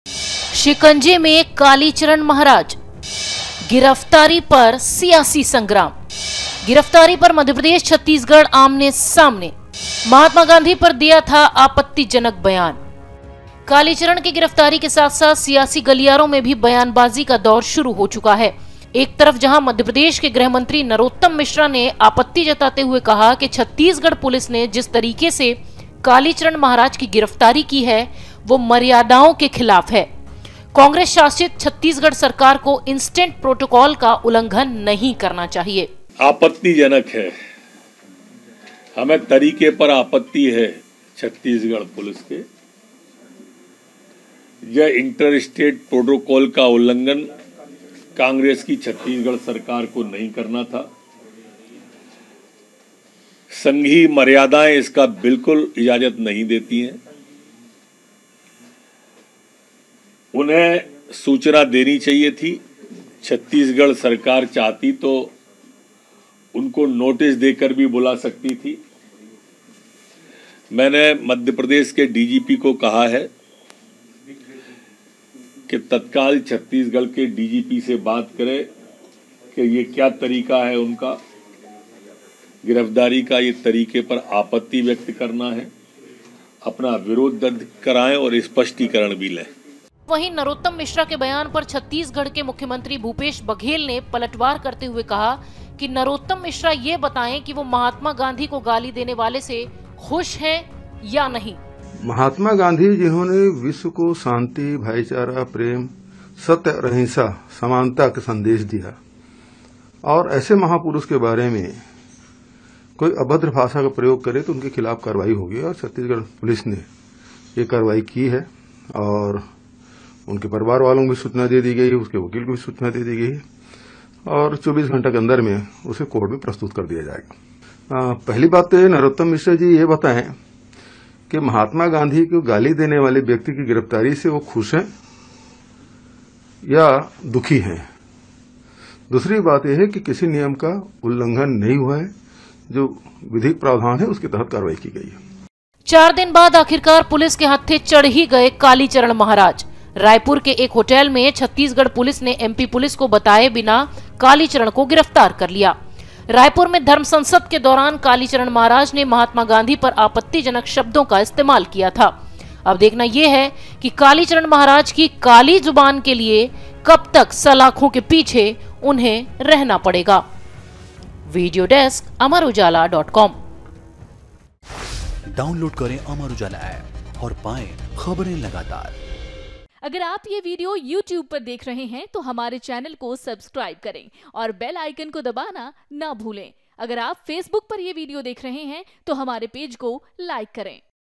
कालीचरण महाराज की गिरफ्तारी पर नरोत्तम मिश्रा ने उठाए सवाल| Kalicharan Maharaj Arrested | MP-CG News
महात्मा गांधी पर टिप्पणी करने वाले काली चरण की गिरफ्तारी के साथ ही सियासी गलियारों में बयानबाज़ियों का दौर शुरू हो चुका है। रिपोर्ट में देखिए राजनेताओं के बीच कैसी चली जुबानी जंग